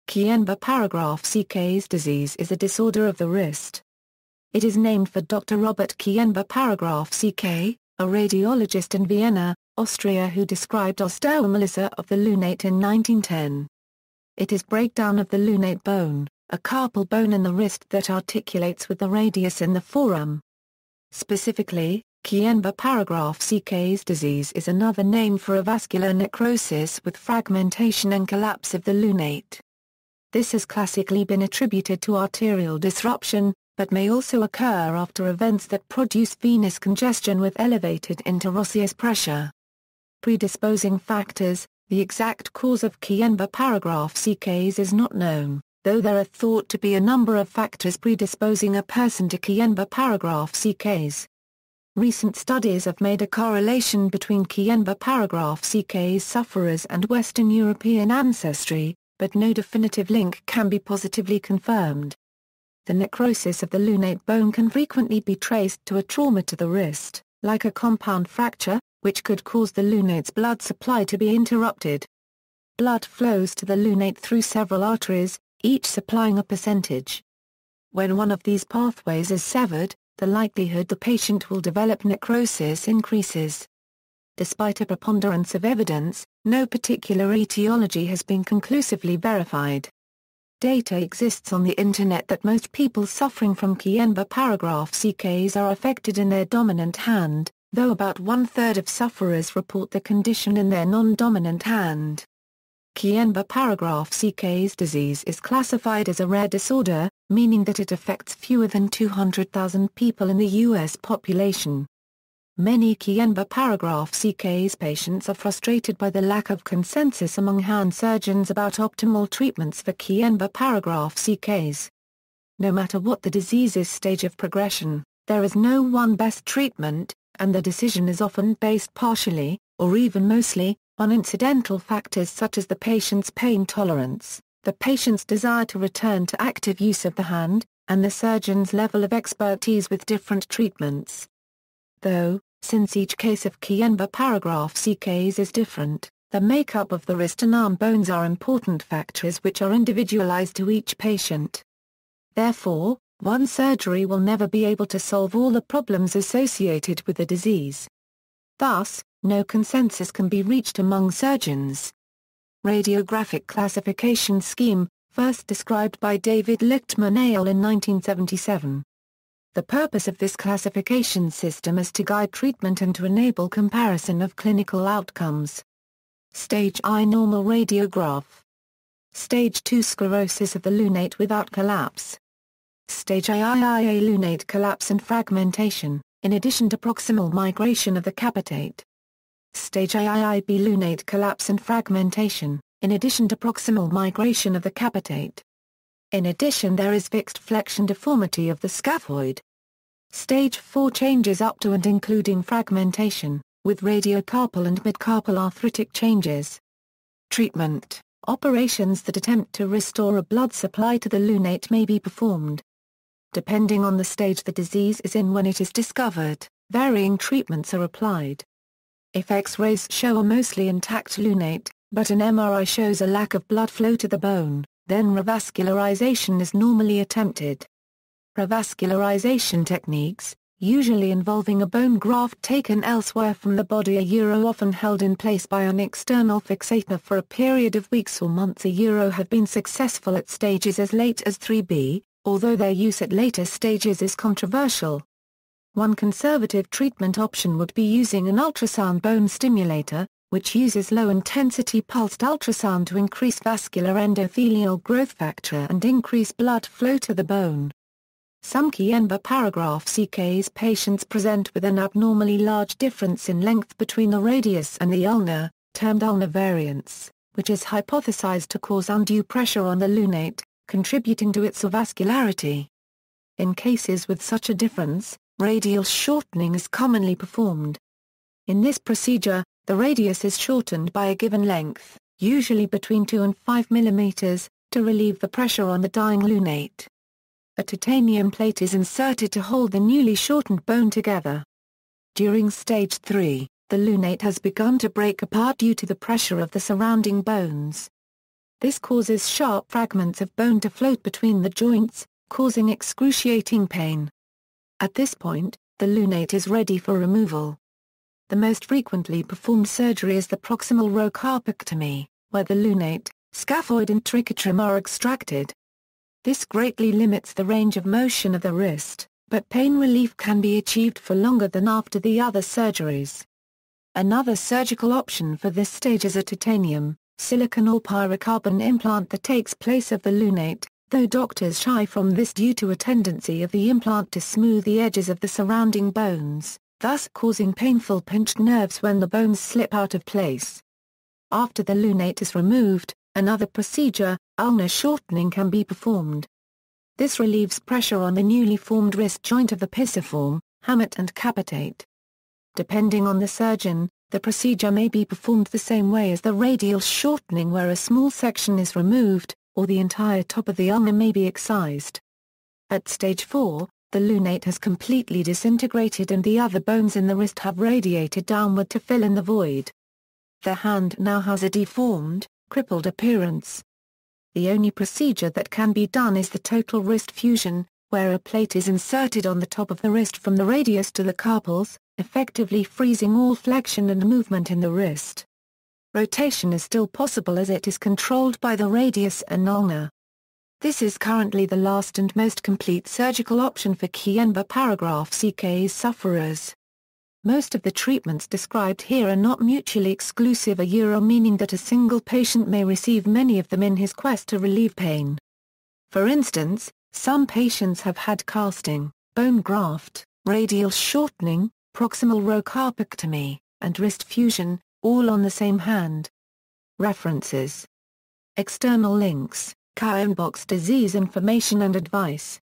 Kienbock's disease
Kienböck's disease is a disorder of the wrist. It is named for Dr. Robert Kienböck, a radiologist in Vienna, Austria who described osteomalacia of the lunate in 1910.
It is breakdown of the lunate bone, a carpal bone in the wrist that articulates with the radius in the forearm. Specifically, Kienböck's disease is another name for avascular necrosis with fragmentation and collapse of the lunate. This has classically been attributed to arterial disruption, but may also occur after events that produce venous congestion with elevated interosseous pressure.